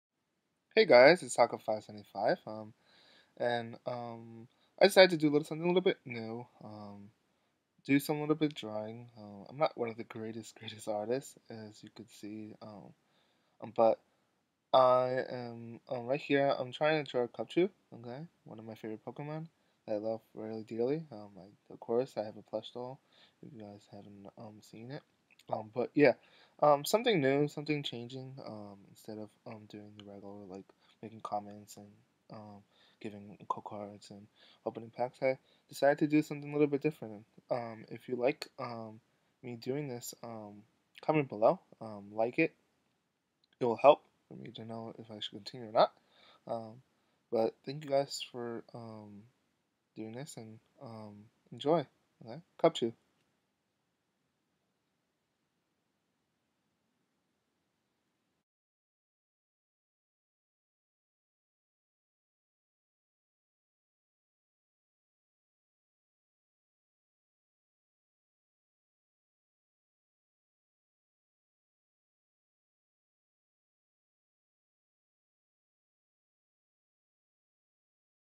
DRAWING CUBCHOO
Hey guy I decide to something different. I wanted to draw a pokemon to make my videos different. I'm not the best artist but I try. Hit like if you like it.